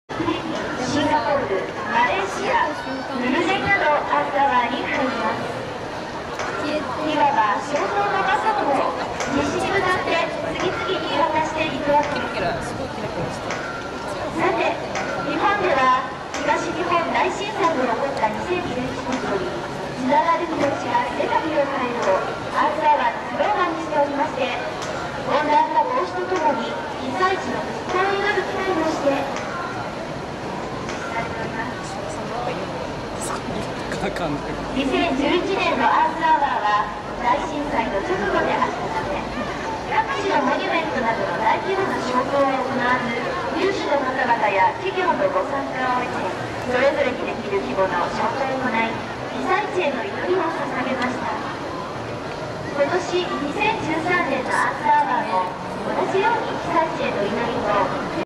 Earth Hour 2013 x Mirrorbowler @ Tokyo Tower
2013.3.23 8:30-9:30